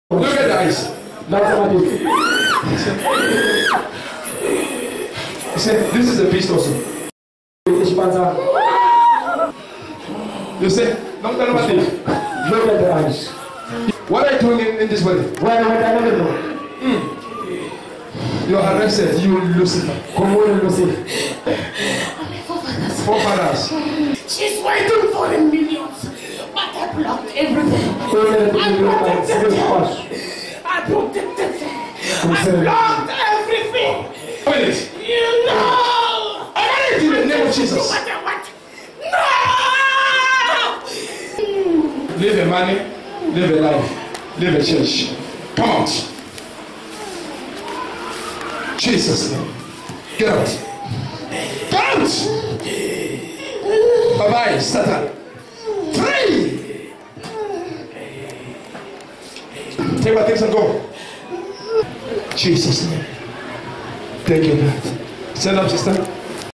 DEMON Possession
The kingdom of darkness is exposed,
Take a look at how the eyes of this young lady appear